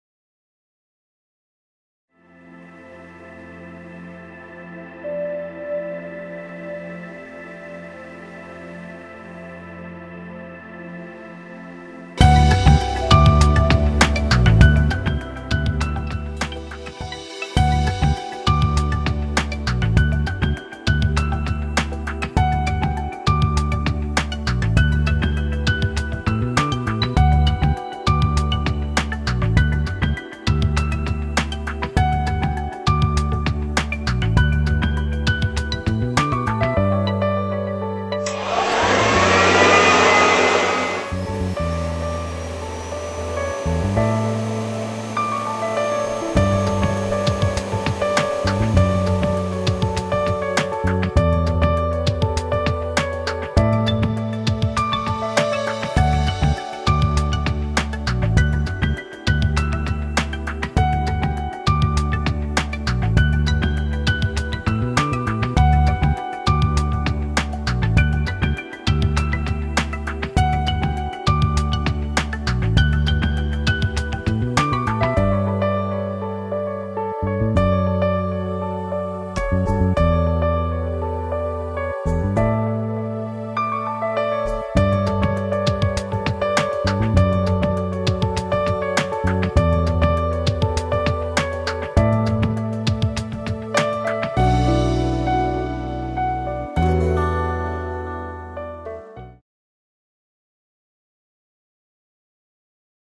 Rowenta Silence Force Extreme [FR]
Power Has Never Been This Silent.
This full sized bagged The Silence Force™ Extreme canister vacuum equals or surpasses the top competitive brands in dust pick up while providing enhanced noise reduction levels.